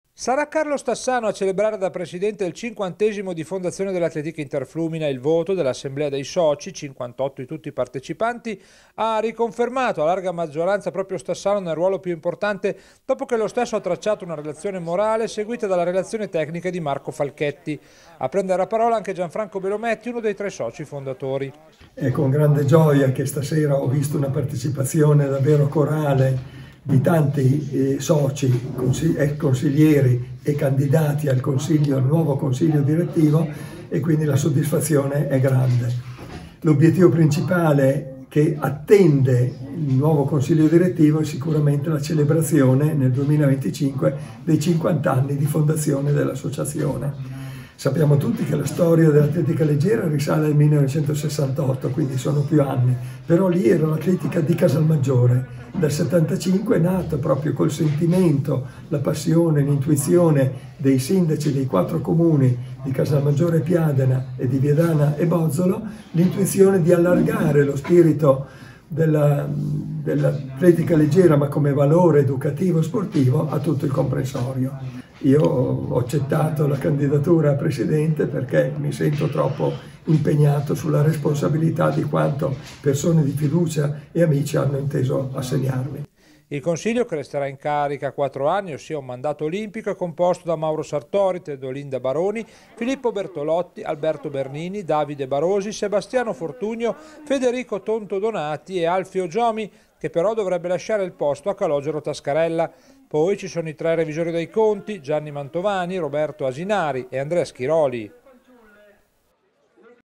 L'Interflumina va in continuità: Stassano confermato presidente, 14 ottobre 2024
Nel 2025 la società festeggerà il 50esimo.